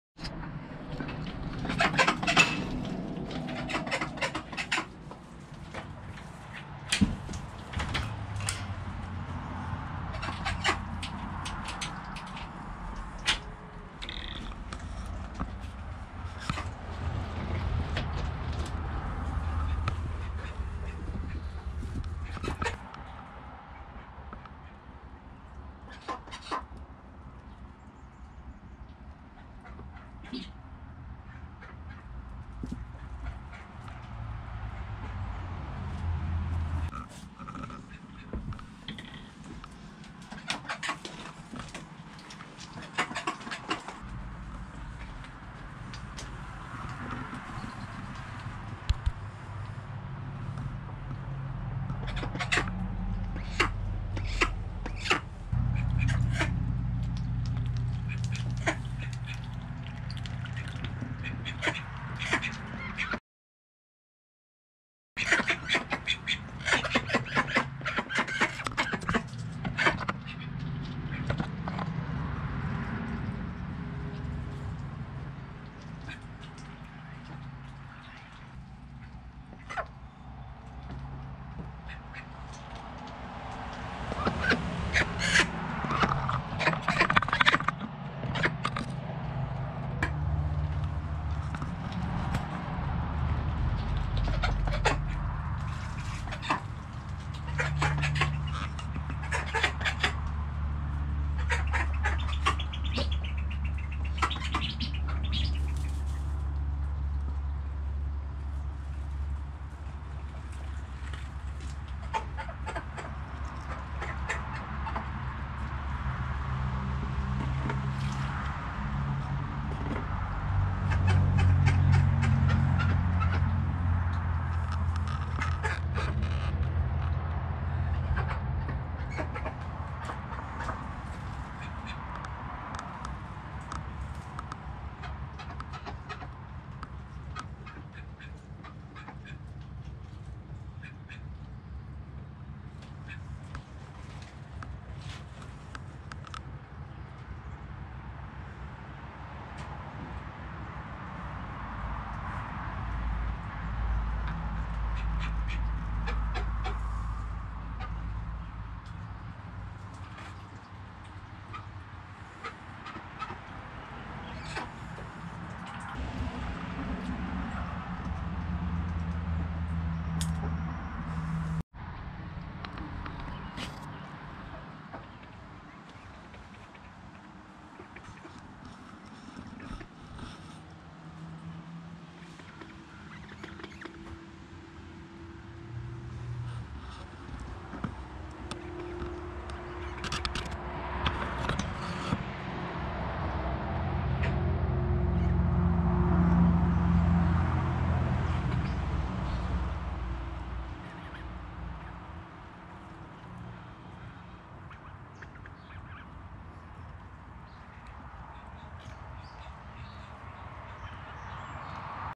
Pen Tour -They Wouldn't let me in... Secret password Apparently
A Look inside Dixie's Suite and the Pen.